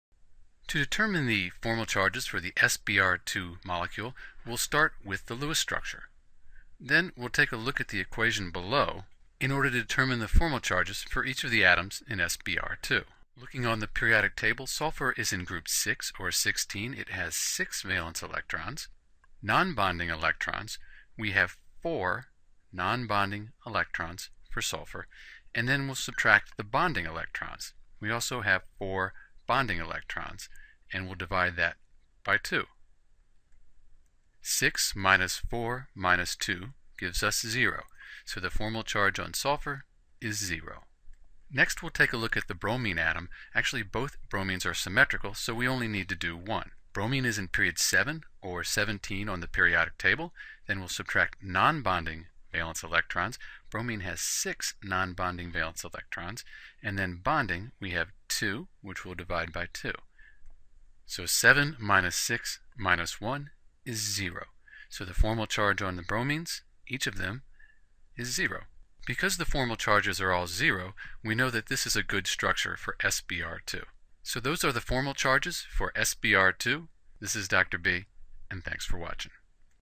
Calculating SBr2 Formal Charges: Calculating Formal Charges for SBr2
In order to calculate the formal charges for SBr2 we'll use the equation

Some things to note about SBr2 Formal Charges:
- Formal charge is not the actual charge.  It is different from the oxidation number.
- The closer the formal charges are to zero the more likely we have the most probable Lewis structure for the molecule.